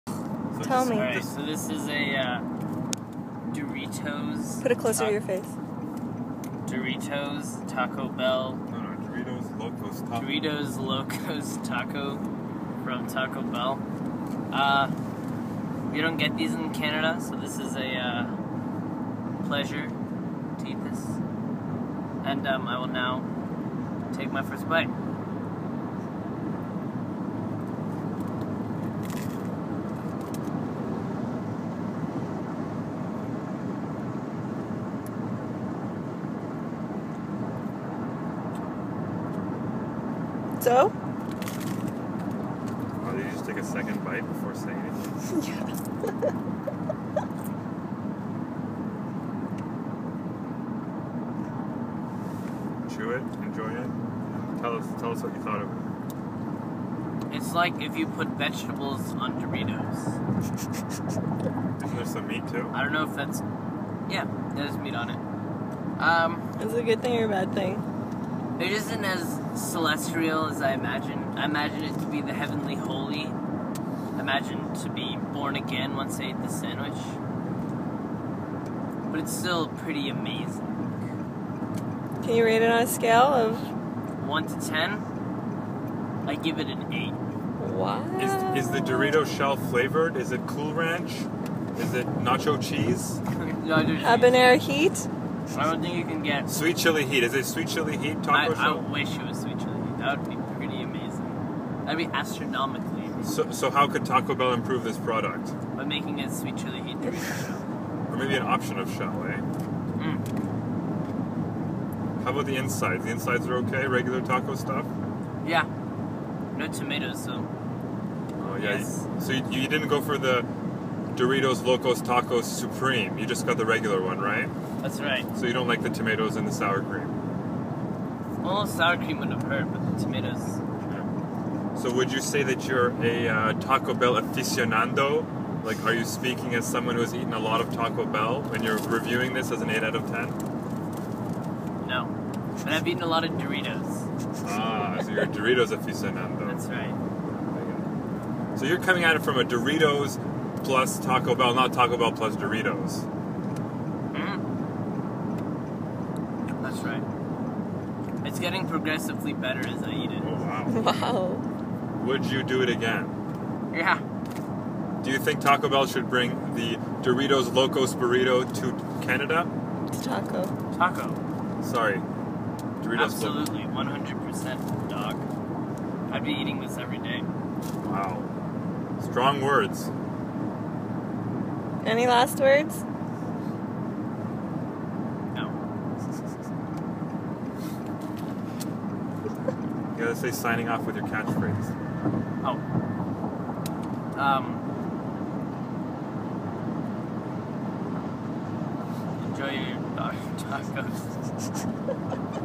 Awkward Canadian Kid Food Reviews - Taco Bell's Doritos Locos Tacos
On the way home from a road trip, we stopped off at Taco Bell to try the new Doritos Locos Taco. Since these aren't available in Canada this was our only chance to give it a go. Watch my awkward brother make up words to describe his experience. Hopefully the first of many, this kids voice needs to be heard across the world. Truly a voice of the generation

Word on the street is that there will be new flavours of Doritos taco shell but no news of Sweet Chilli Heat just yet.